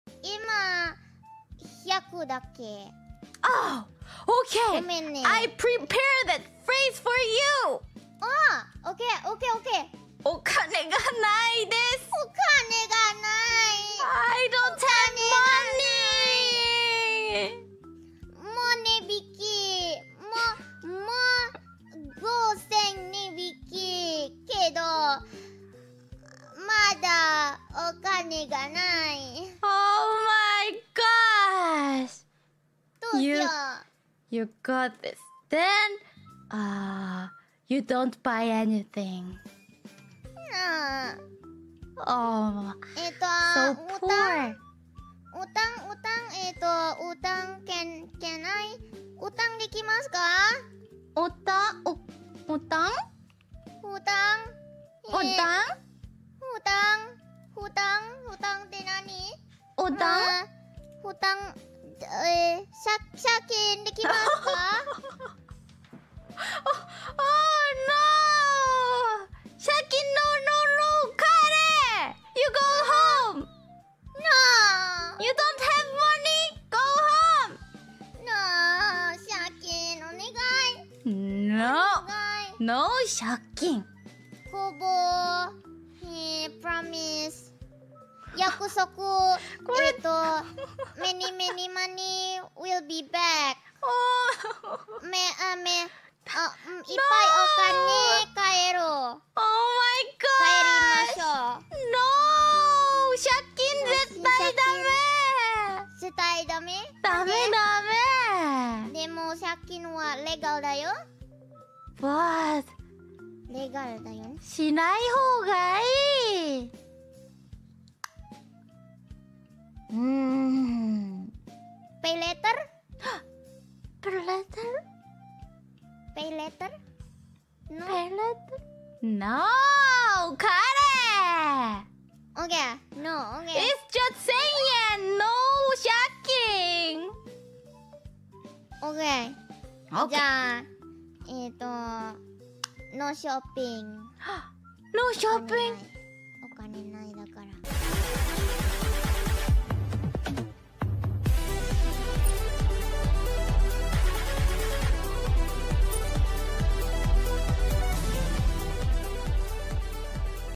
Kanade Tells Kobo Not To Get Into Debt Because She Wants To Buy Something But Has No Money
Stream Source :
Kanade 【オフコラボ】Kobo先輩がうちにきちゃ！！【音乃瀬奏・Kobo Kanaeru】hololivedev_is

HololiveID 3rd Gen Member